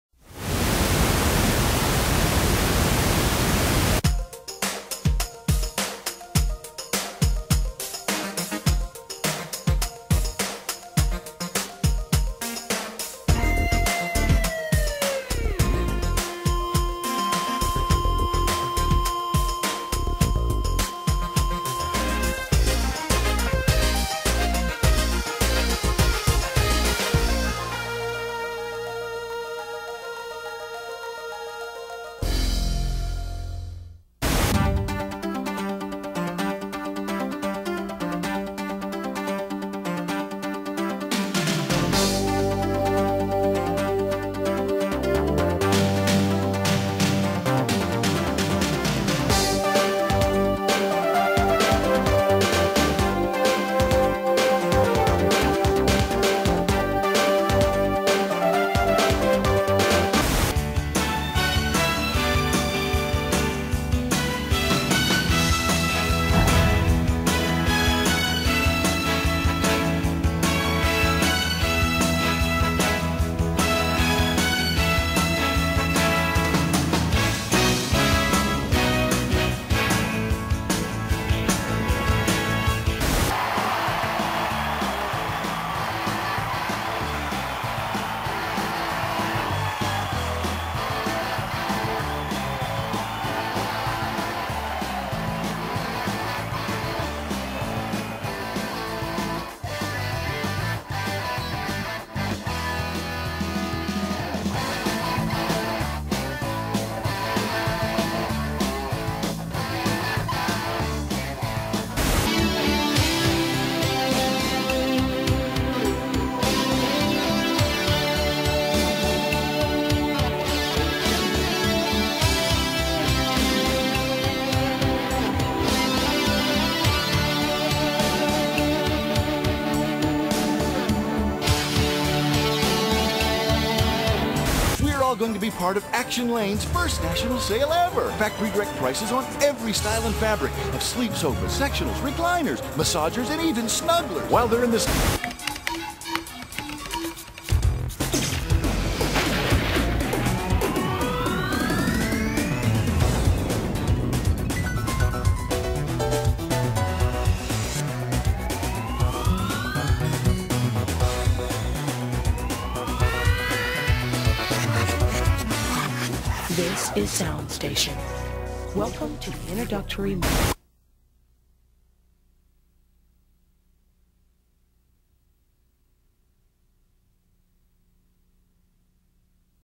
Facility Demo PVS SPEER INTERNATIONAL
I produced this facility demo showcasing the wide variety of projects PVS SPEER undertook...In addition to producing the final piece I was the Director or TD on the majority of the programs...the facility in Nashville is now called Northstar Studios